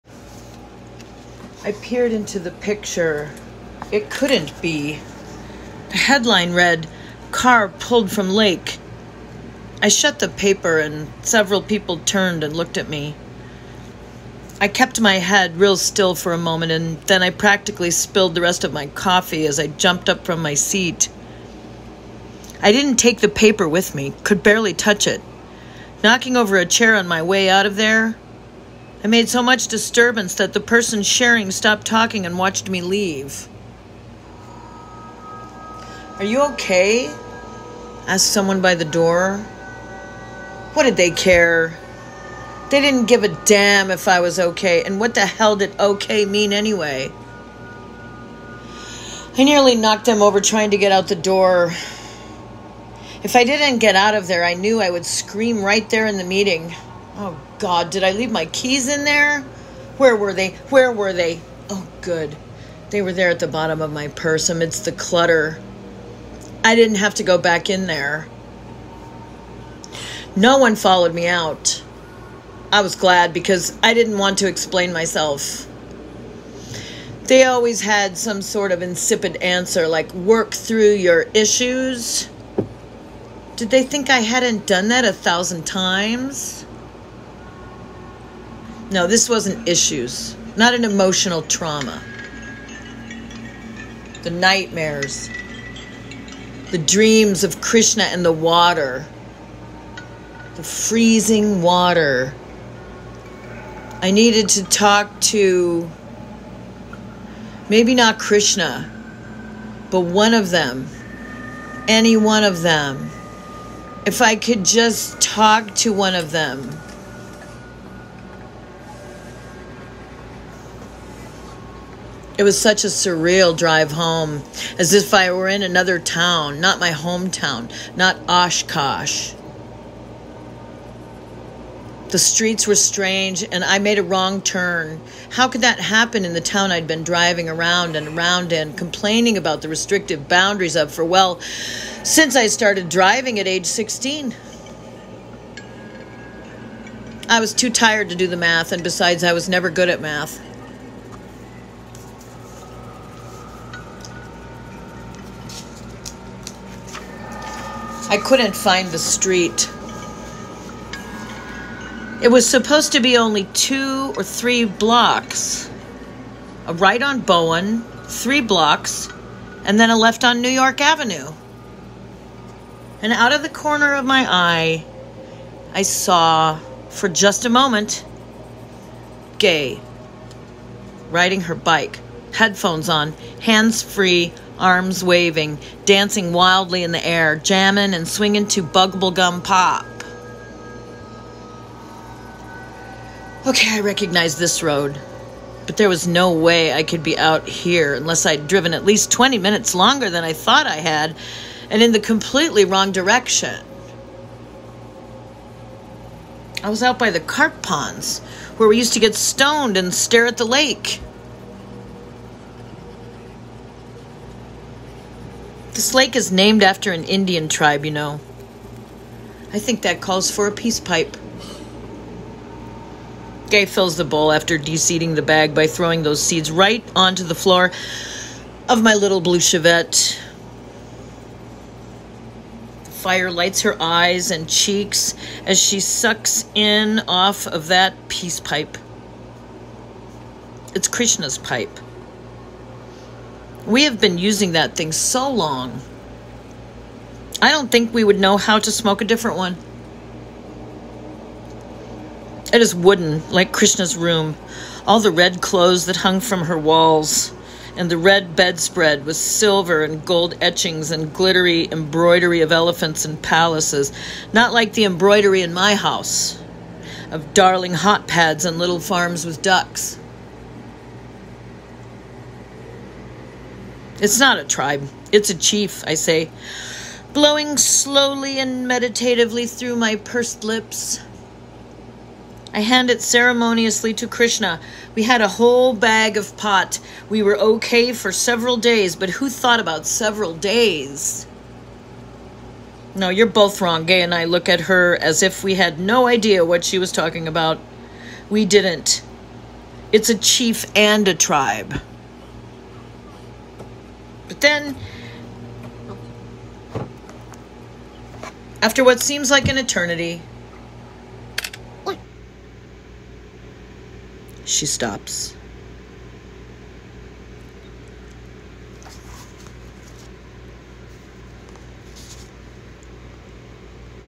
Drawing the Library From Memory (Reading Excerpt From The Dark Lake)
Dark Academia

Reading Excerpt From The Dark Lake

Description
Something is wrong with Jane.

It's not that she still lives with her parents in Oshkosh, Wisconsin, although she's well into her 30s. It's not that she can't keep a job, stay sober, or function as an independent adult. It's something else. Something she can't quite remember.

There was a party. And a car. And the lake.